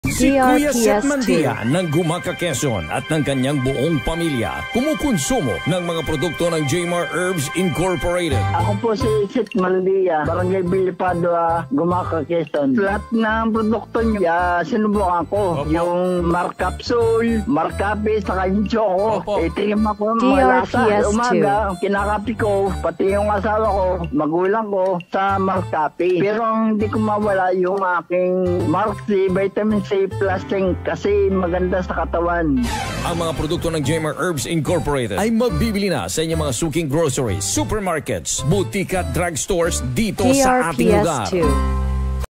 JMAR Herbs Products Radio Ad 2022-2024 42s (Testimonial Version 3) [ST]
Ito po ang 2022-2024 Radio Ad ng JMAR Herbs Products. (Testimonial Version 3)

DRPS Two 8557: JMAR Herbs Products Radio Ad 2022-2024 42s (Testimonial Version 3) [ST]